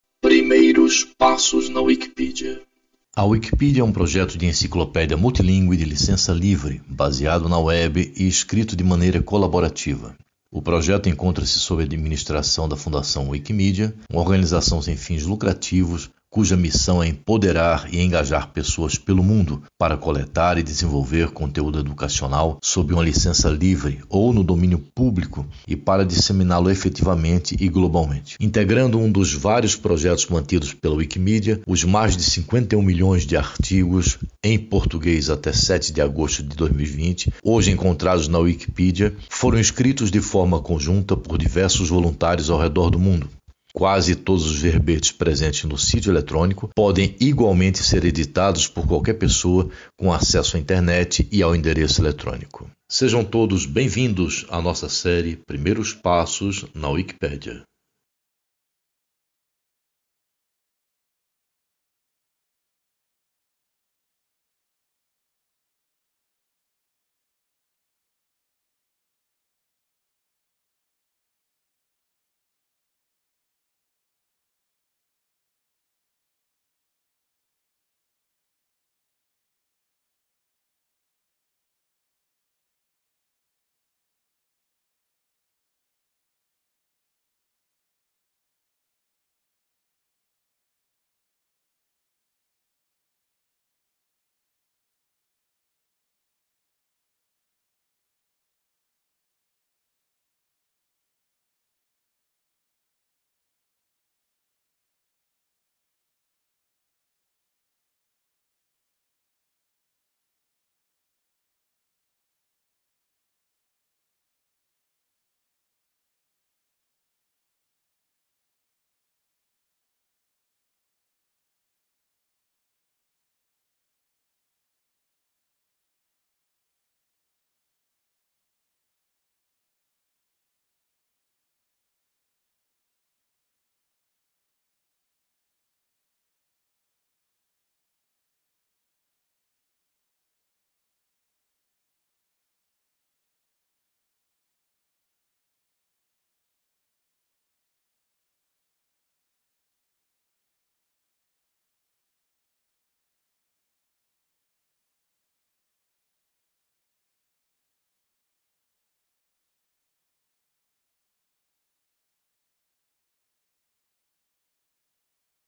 O que é Wikipedia ?
A Wikipédia é um projeto de enciclopédia multilíngue de licença livre,[5][6] baseado na web e escrito de maneira colaborativa.[6] O projeto encontra-se sob administração da Fundação Wikimedia,[7] uma organização sem fins lucrativos cuja missão é "empoderar e engajar pessoas pelo mundo para coletar e desenvolver conteúdo educacional sob uma licença livre ou no domínio público, e para disseminá-lo efetivamente e globalmente".[8] Integrando um dos vários projetos[5] mantidos pela Wikimedia...